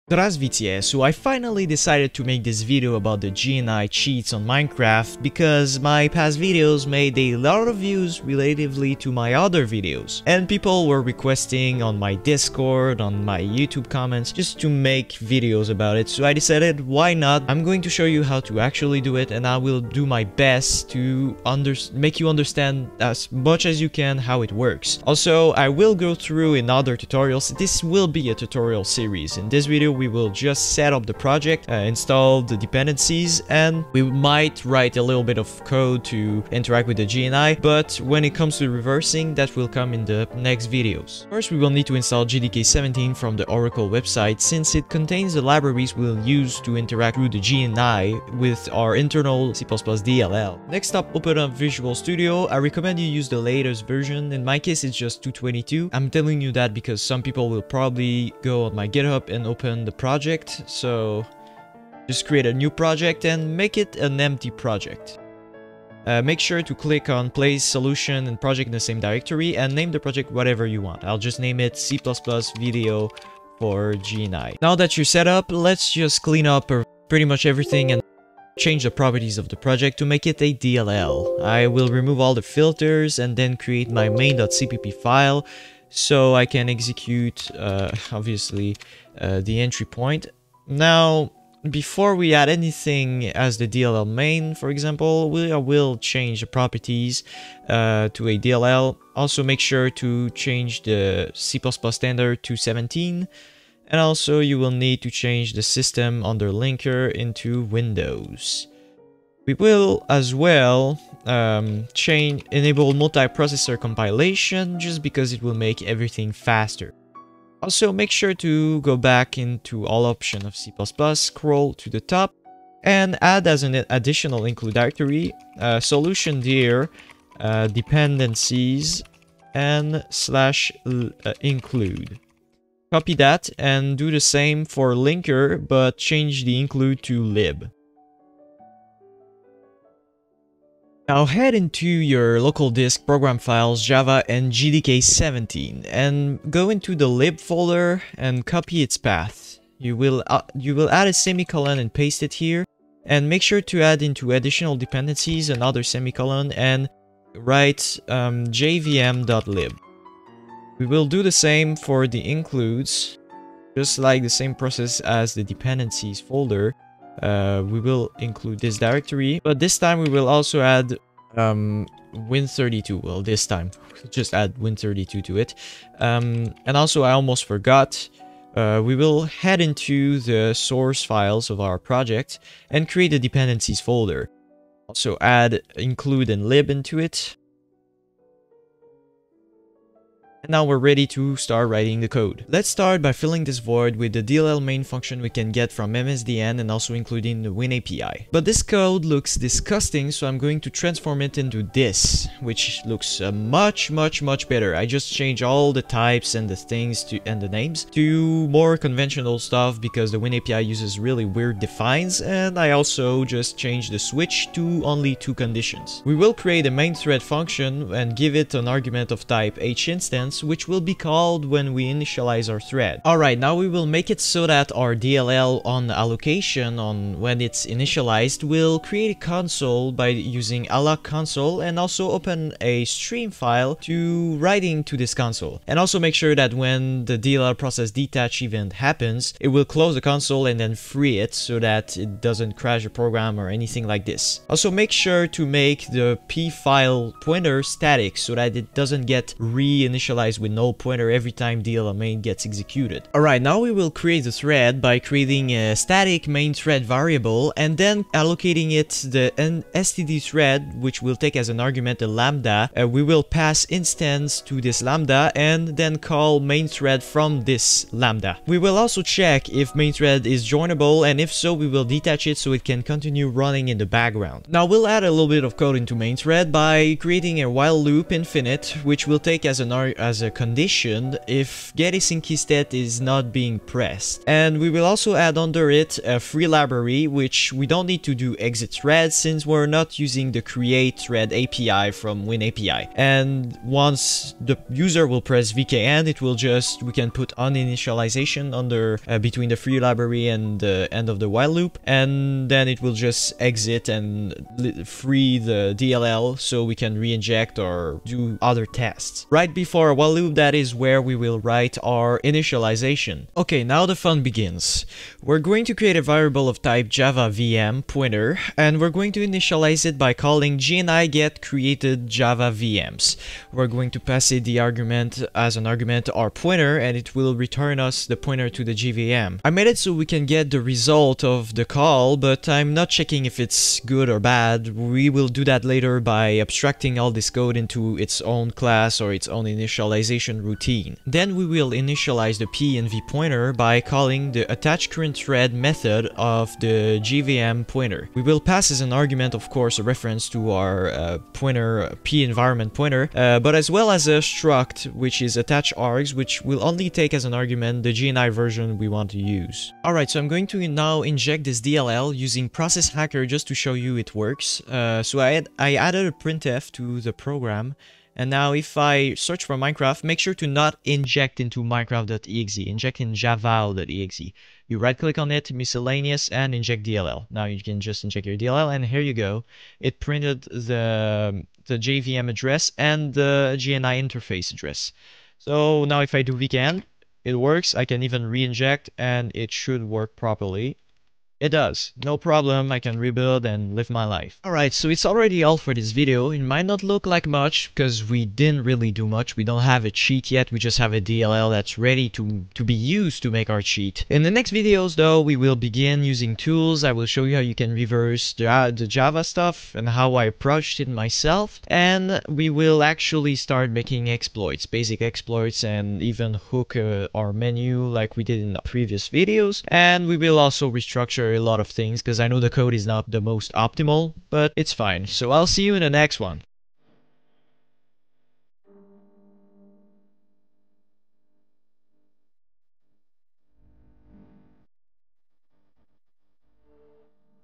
Intro - Unlock the Secrets of a Minecraft JNI Hacked Client
In this series, I'm going to teach you how to hack a Minecraft JNI client. This JNI client will allow you to do things that are normally impossible, like move and access blocks that are normally off-limits. JNI is far from being well documented, so it seems normal that many struggle to traverse this jungle, my goal is to make this issue obsolete.

⏰Chapters⏰
00:00 - Intro
00:40 - Install JDK
00:50 - Create and Configure Project
03:12 - Writing Code
06:00 - Injecting DLL
06:34 - Outro

Shout out to @kapitcheGHT for the thumbnail ⭐